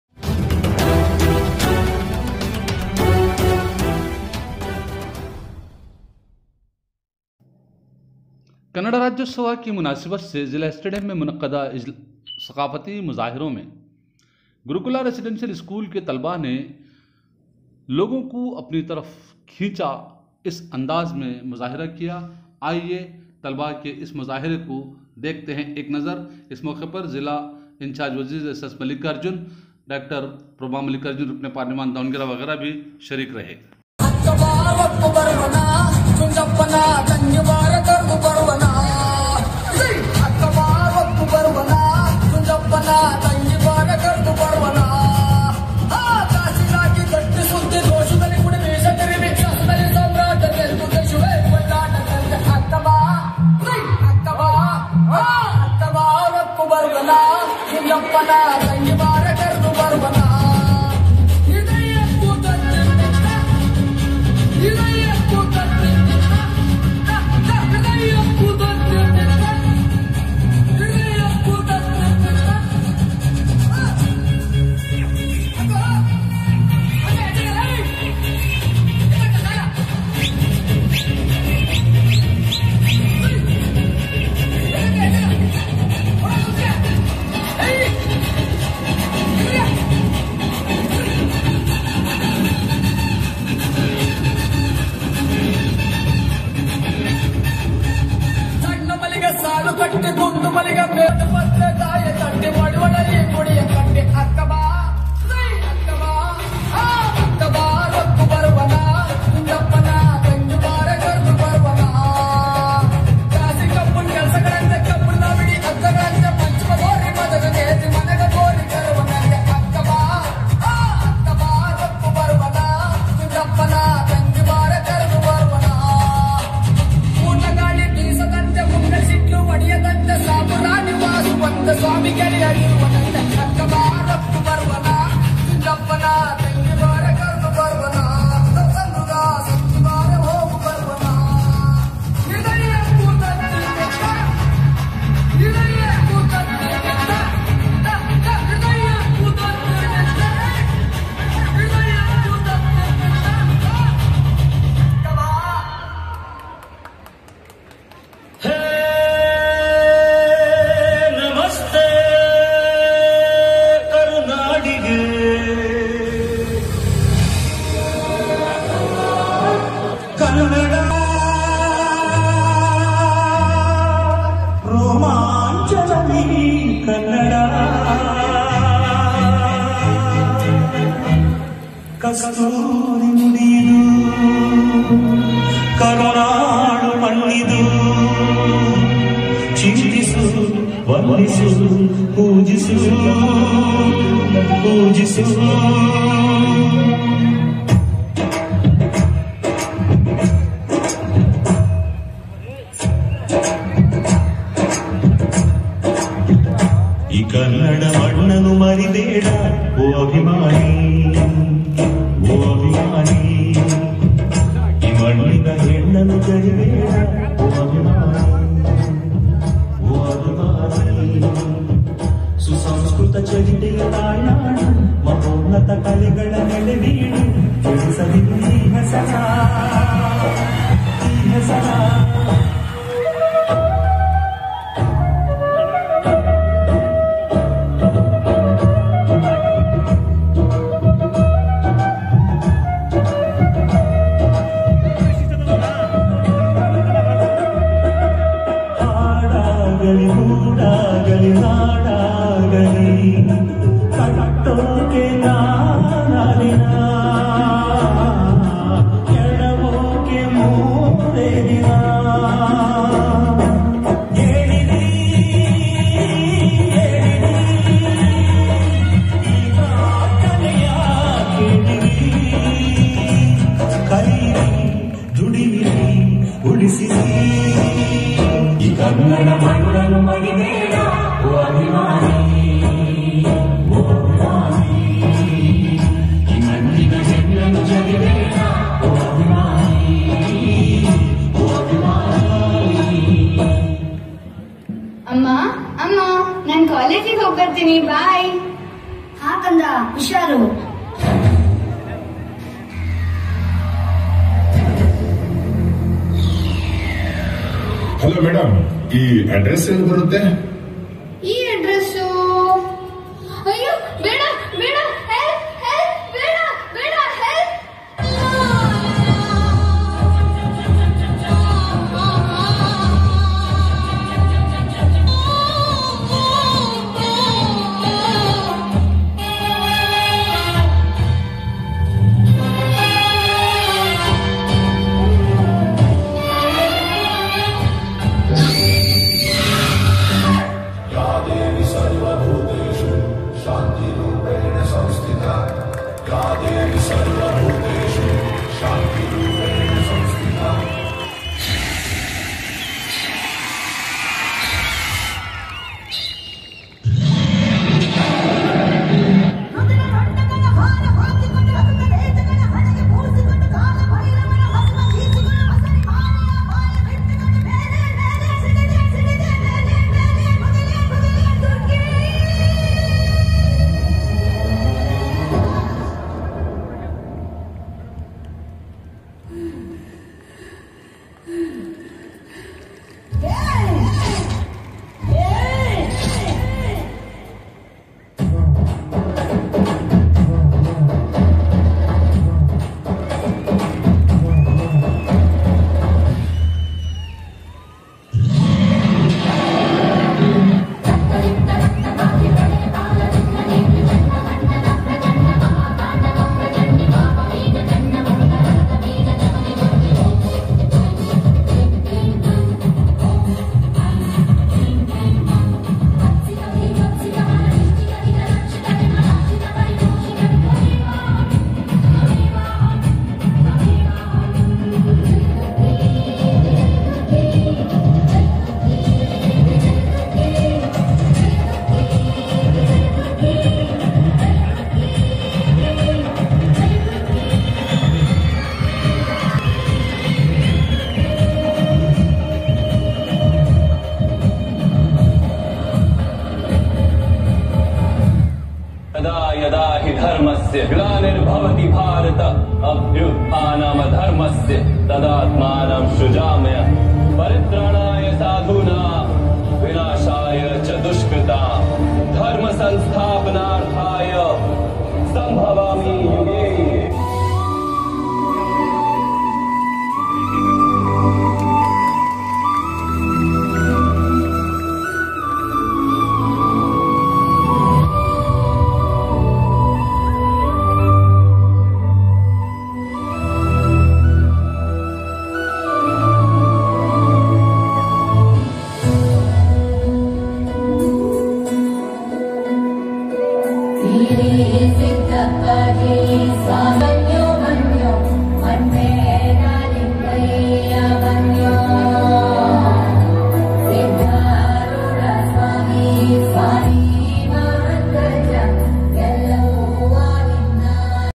GURU KULA RESI SCHOOL TALBA KE ZARIYE ZILLA STADIUM ME SAKFAFATI PROG KA MANZER PART1#JANTA KI PUKAR
© 𝗔𝗹𝗹 𝗰𝗼𝗽𝘆𝗿𝗶𝗴𝗵𝘁𝘀 𝗮𝗿𝗲 𝗿𝗲𝘀𝗲𝗿𝘃𝗲𝗱 𝘁𝗼

      " Janta Ki Pukar "

             " ▶️ Janta Ki Pukar "

For Latest Videos & Updates